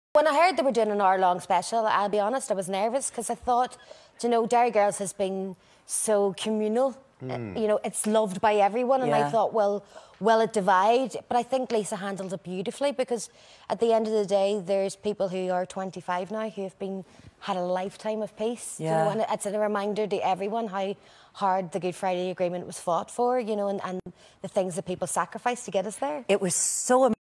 Tara Lynne O’Neill on the impact of Derry Girls & the new generation of peace
“Derry Girls has been communal”

Tara Lynne O’Neill reflects on her time on Derry Girls.